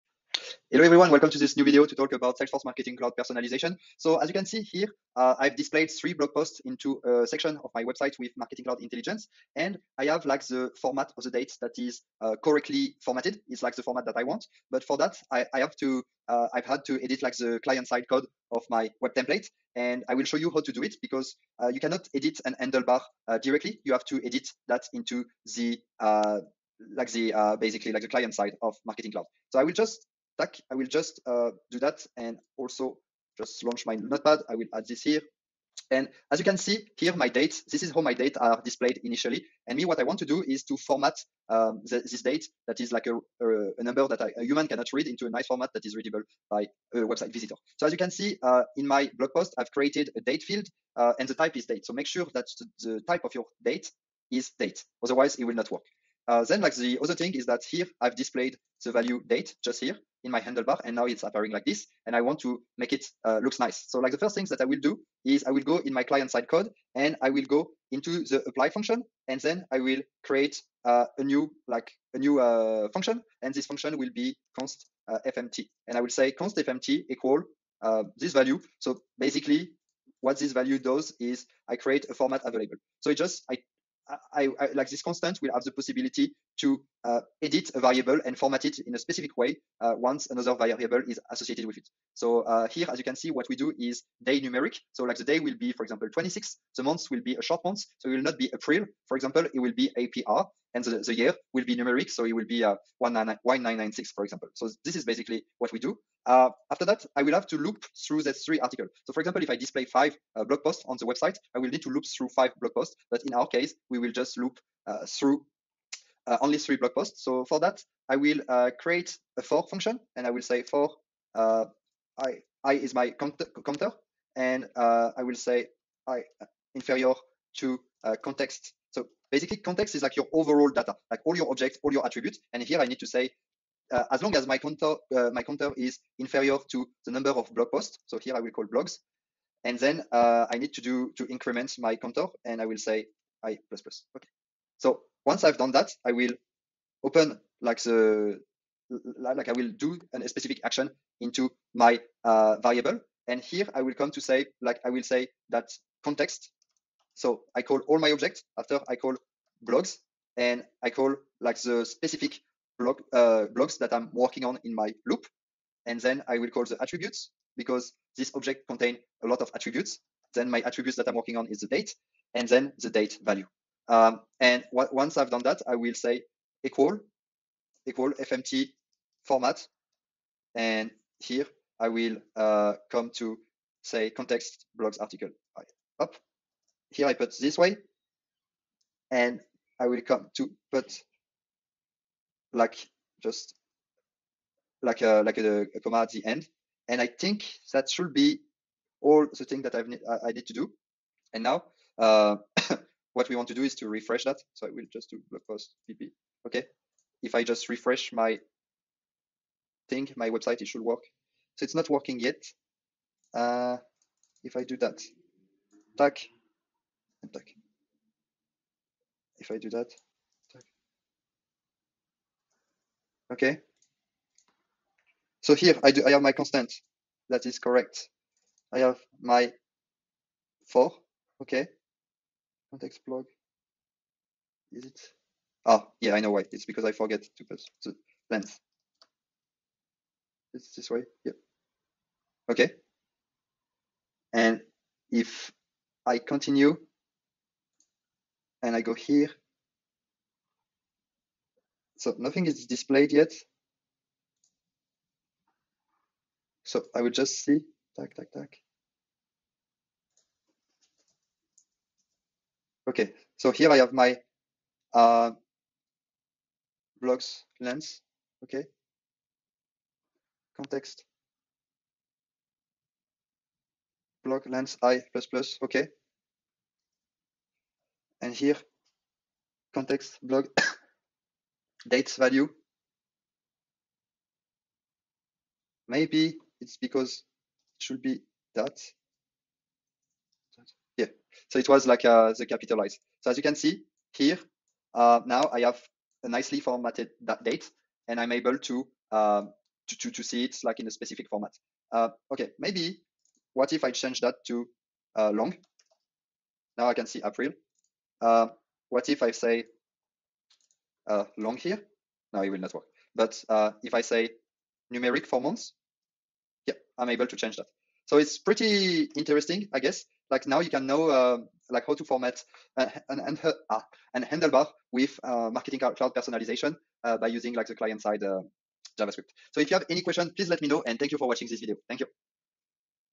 Marketing Cloud Personalization | How to edit HandleBars
In Marketing Cloud Personalization, you can use Handlebars in the Handlebars Tab of the Template Editor to display content from the Object Catalog (e.g. Products, Articles). However, the function registerHelper cannot be used in Marketing Cloud Personalization, so code needs to be added in the ClientSide Tab of the Template Editor to proceed or format Handlebars variables.

This short tutorial will demonstrate how update the client-side code section of the Web Template in order to overwrite a data attribute into the context object for use in the handlebars template.

Kind Regards,
Arthur